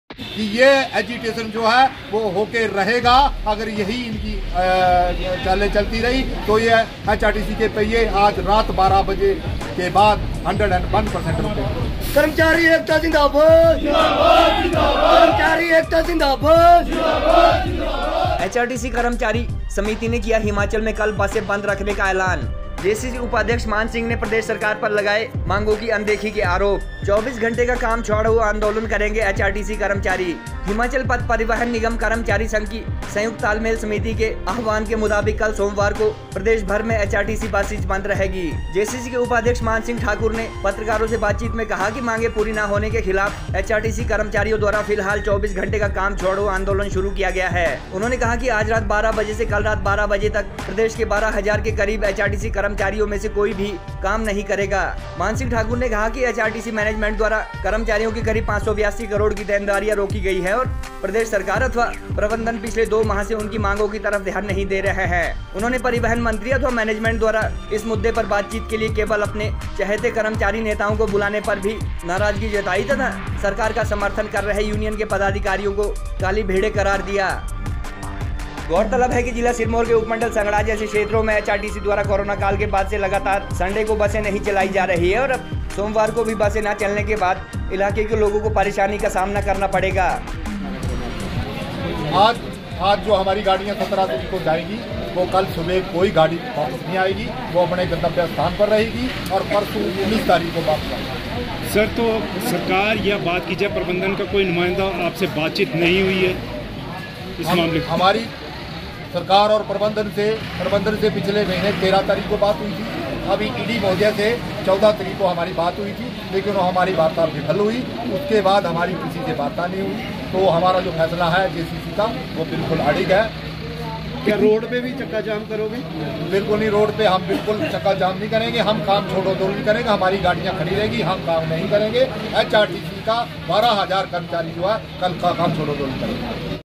हिमाचल में कल HRTC Buses बंद रखने का ऐलान
हिमाचल में आज रात 12PM से 24 घंटे परिवहन निगम के बसें बंद करने का ऐलान HRTC JCC ने कर दिया है। मांगे पूरी न होने पर व वार्ता के लिए न बुलाने के मामले मे सोमवार को सभी बसें खड़ी करने का संदेश Drivers व परिचालकों को दे दिया गया है। संयुक्त समन्वय समिति उपाध्यक्ष मान सिंह ठाकुर ने कहा की, कल रात 12 बजे तक बसें खड़ी कर 1 दिन की हड़ताल की जा रही है।